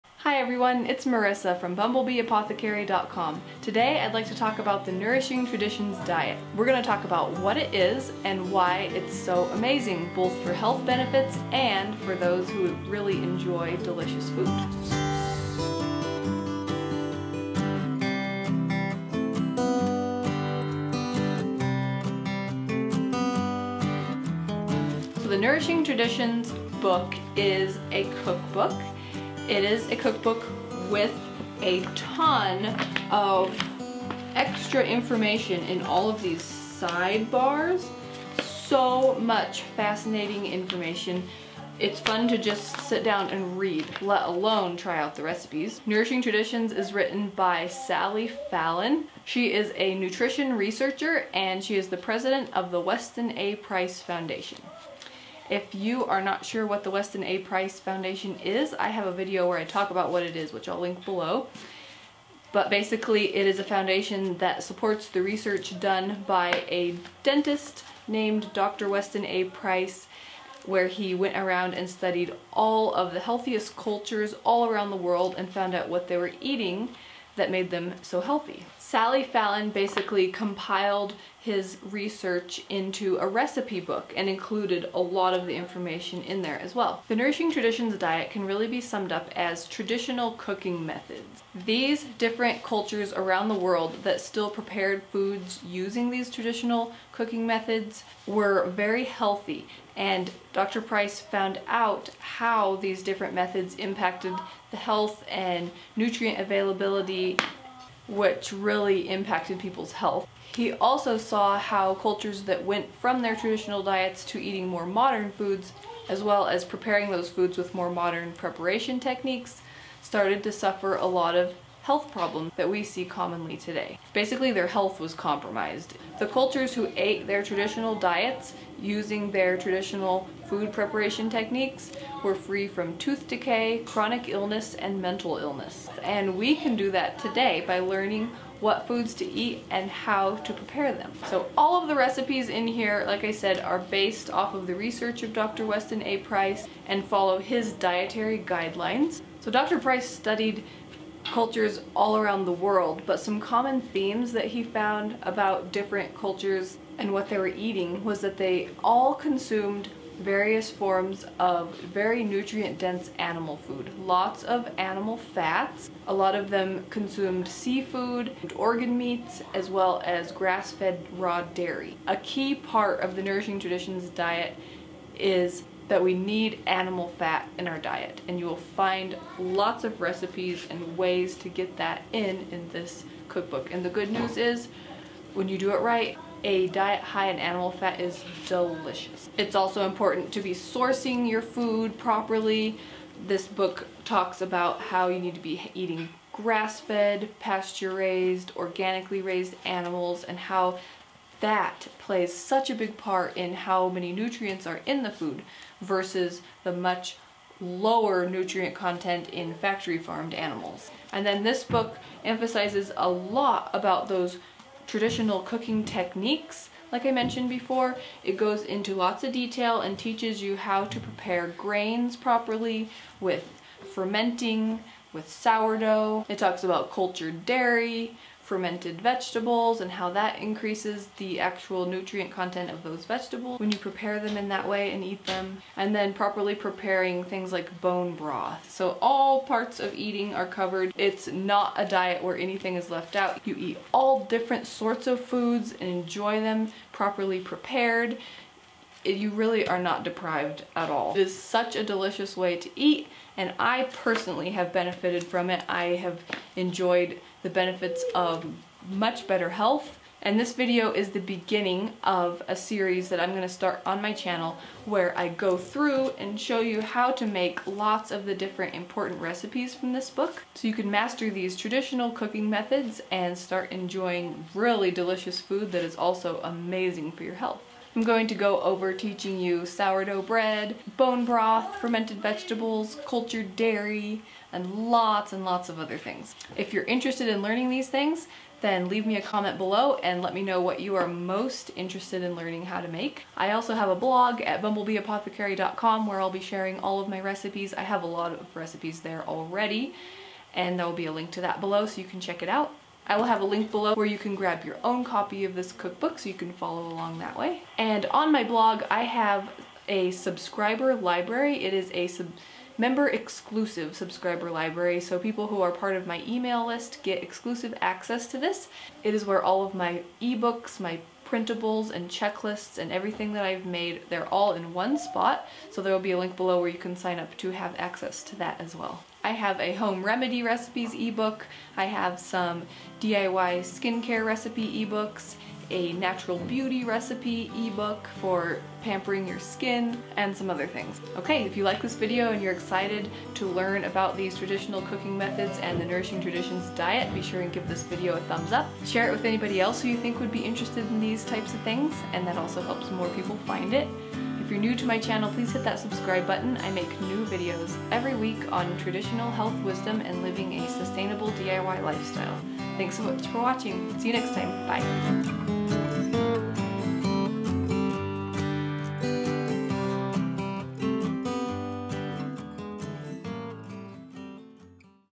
Nourishing Traditions Diet | TRADITIONAL COOKING METHODS | Bumblebee Apothecary
NOURISHING TRADITIONS DIET | TRADITIONAL COOKING METHODS // What is a nourishing traditions diet? How does it improve health? Let's take a look at this popular way of eating, and find out why it's so famous.

-Marisa 🐝